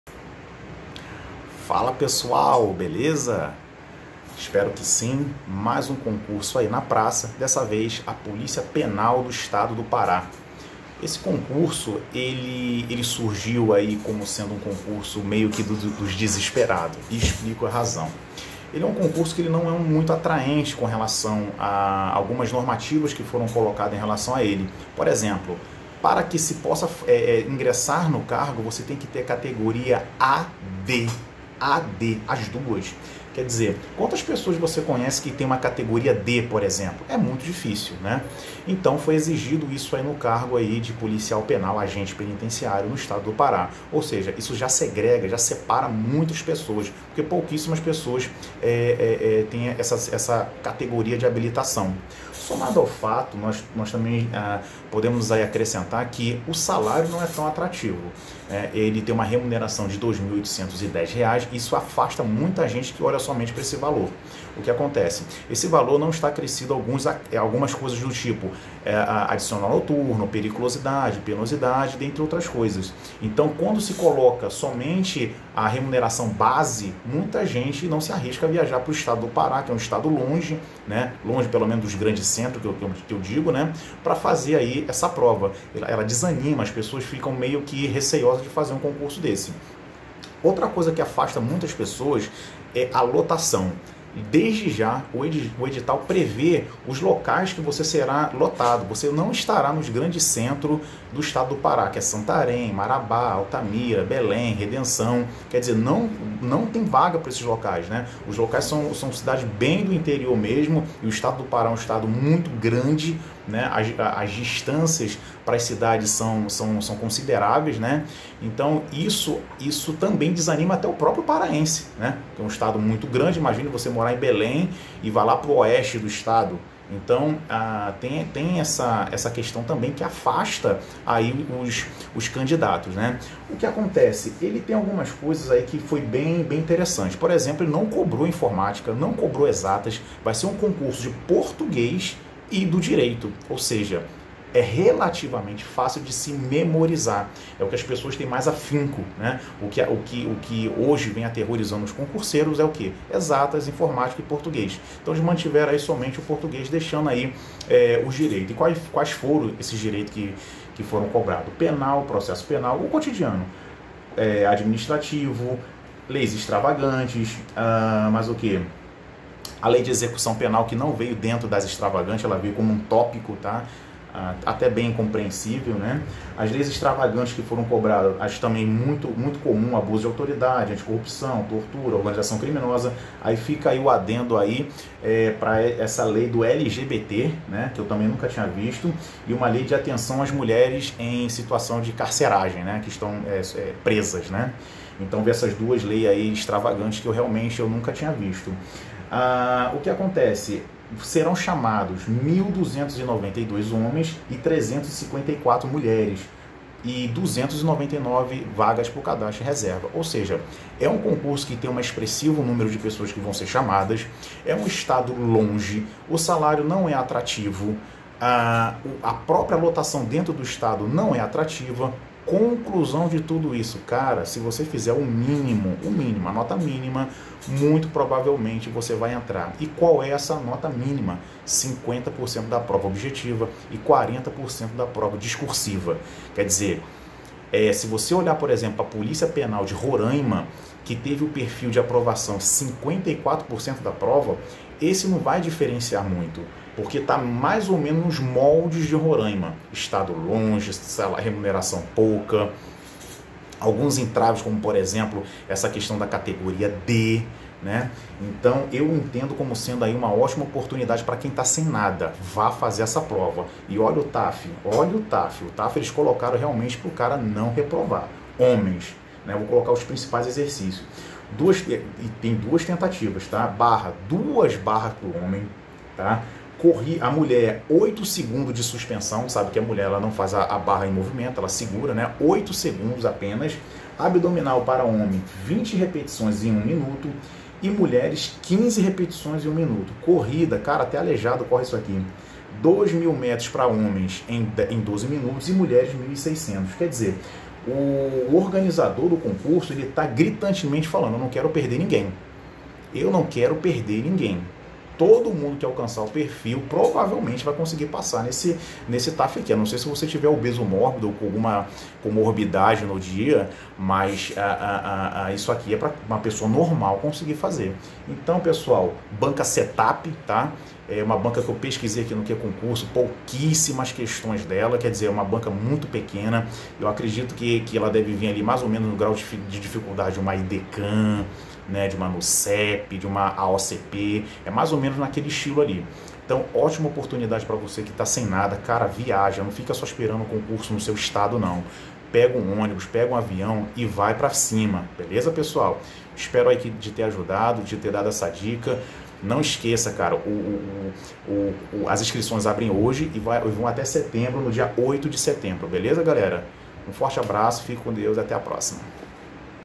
POLICIA PENAL DO PARÁ: Uma ótima oportunidade para quem não tem nada De cargo público !
Concurso policia penal do PARÁ, ótima oportunidade de garantir algo !!!